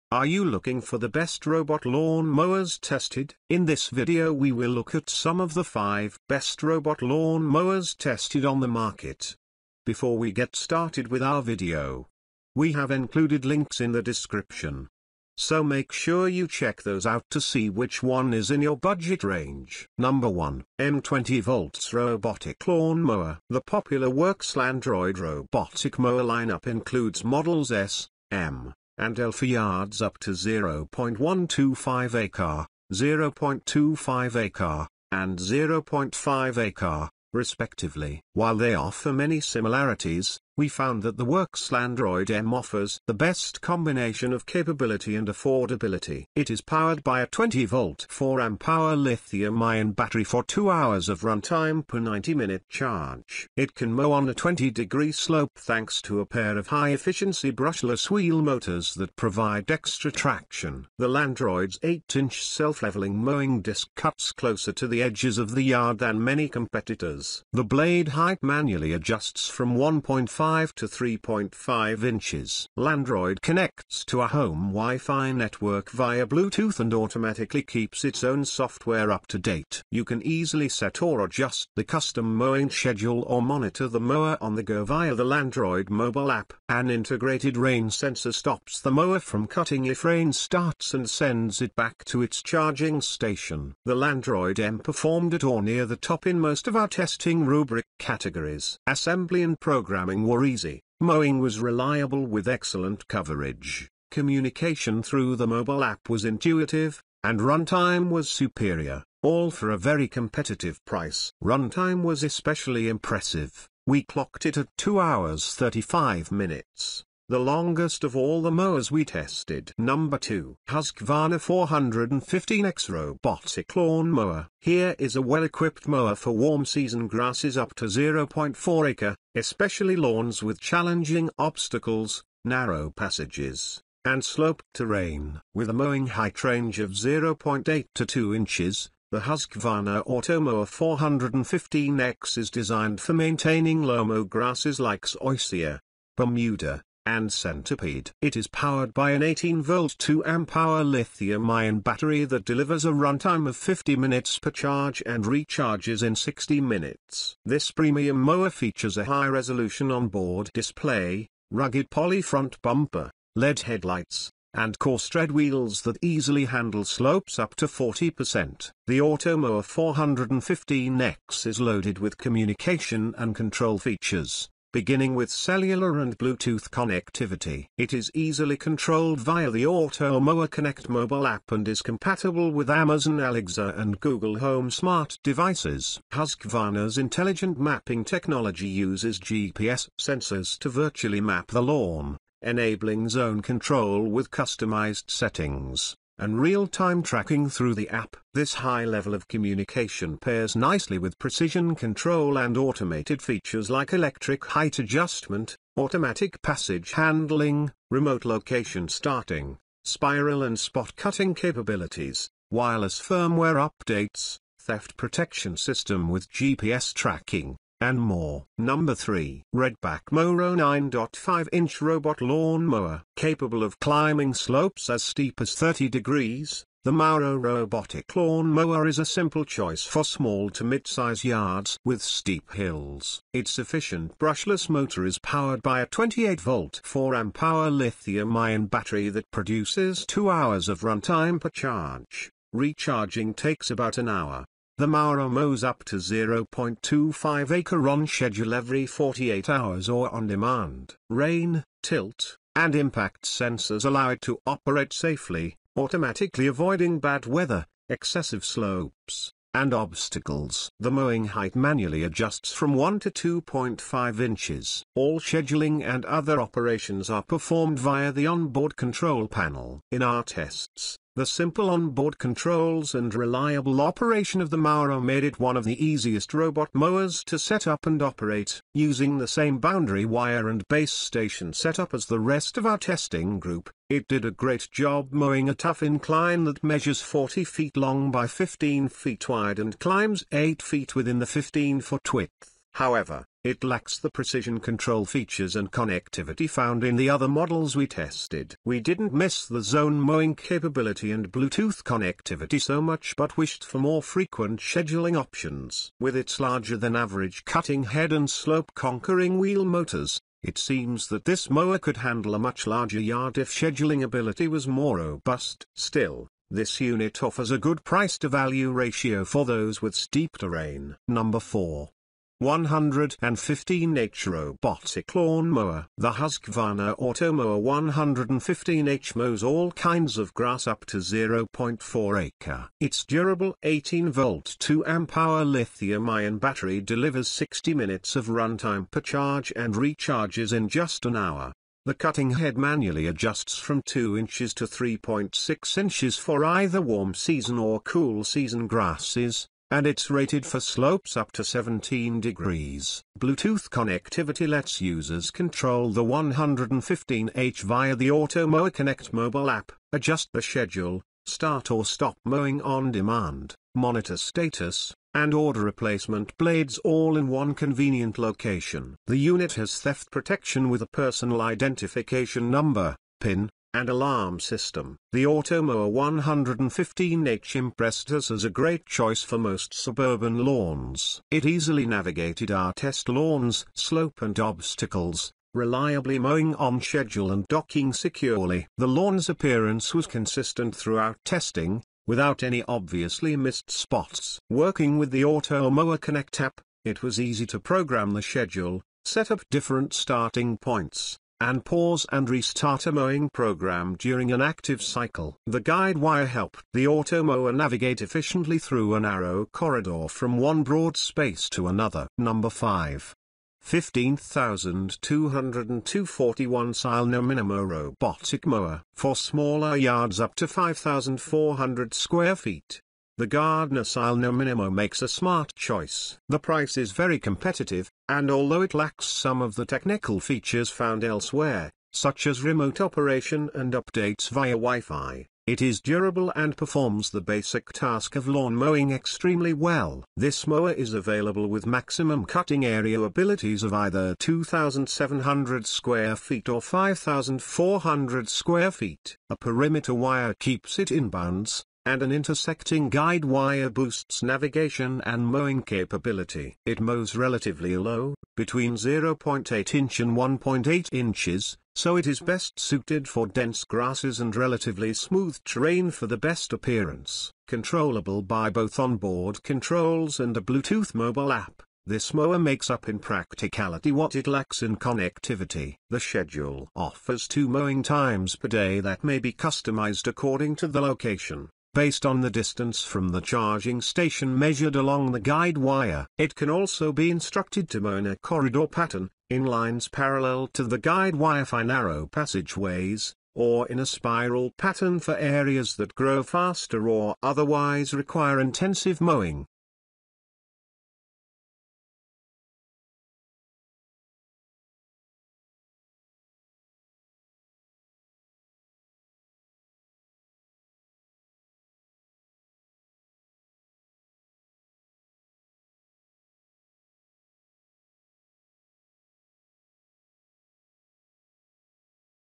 TOP 5 Best Robot Lawn Mowers Tested of 2023 Tested and Reviewed
📌Best Robot Lawn Mowers Tested Link📌:
1. M 20V Robotic Lawn Mower.

2. Husqvarna 415X Robotic Lawn Mower.

3. Redback MowRo 9.5-Inch Robot Lawn Mower. -13%

4. 115H Robotic Lawn Mower.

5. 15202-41 Sileno Minimo Robotic Mower. -14%

In this video, we listed the Best Robot Lawn Mowers Tested that are available on the market for their true quality, actually, I tried to make the list based on their popularity quality-price durability user opinions and more.